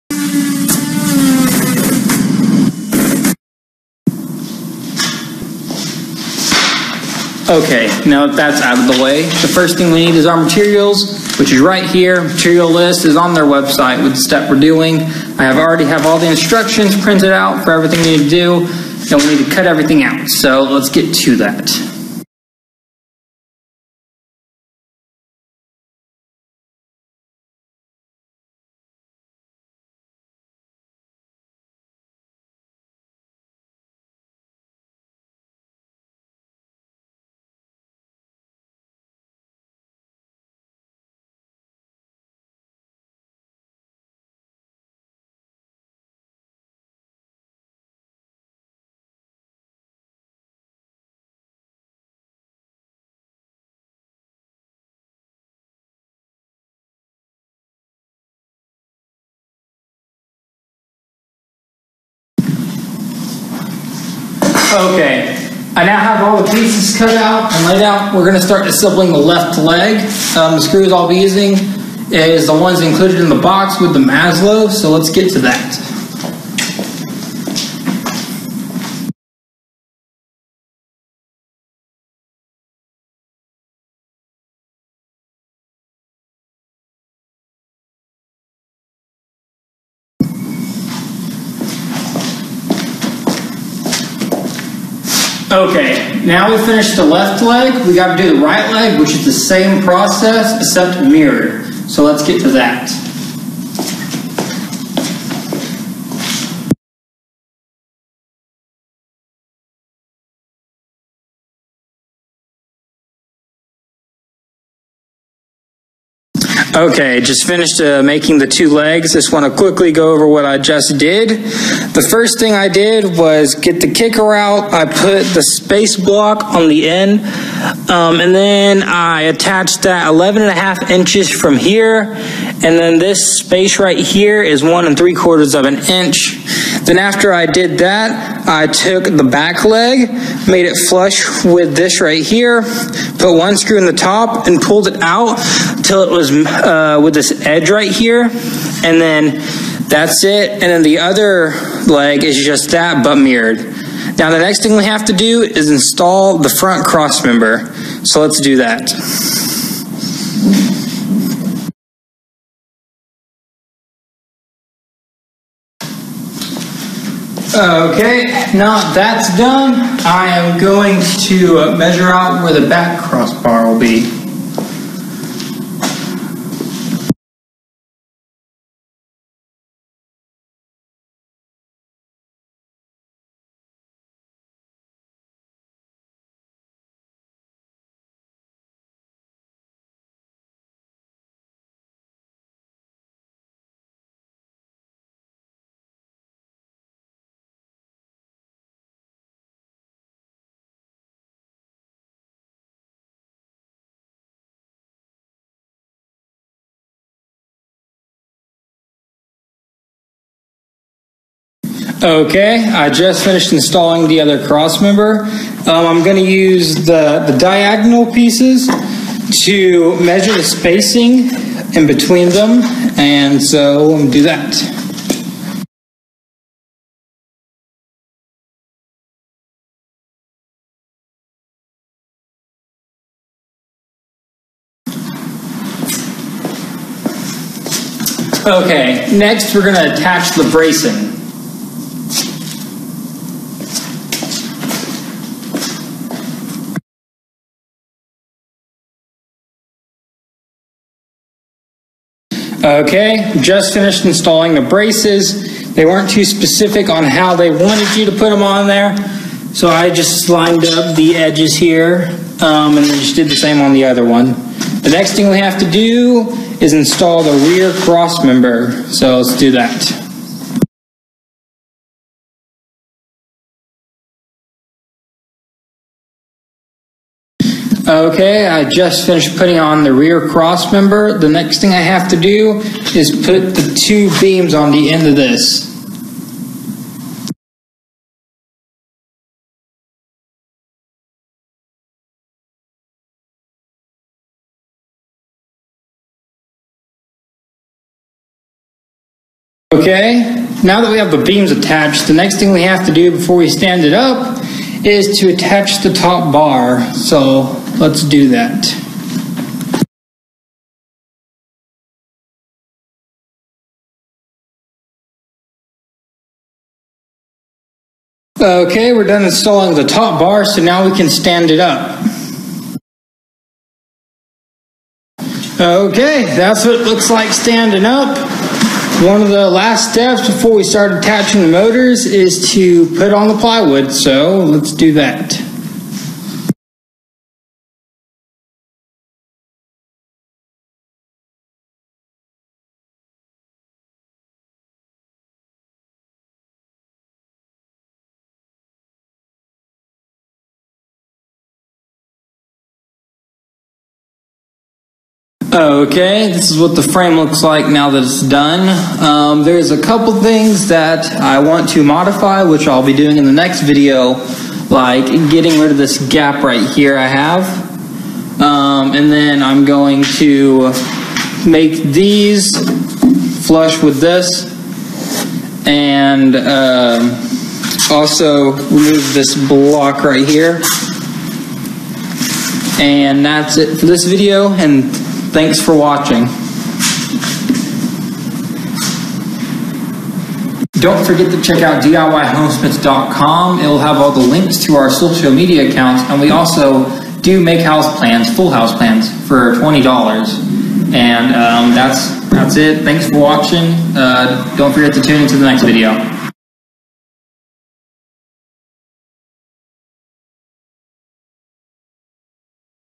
Maslow CNC Setup: Step #4 Part 1 Building the Default Frame
This is the fourth video in a series covering the setup of the Maslow CNC. The machine cost $350 and can be purchased

The steps in this video series are the same steps listed on the Maslow web site.

For more info on us and our other projects, check out our

In our store on diyhomesmith you can find ready to build house plans starting at $20ea.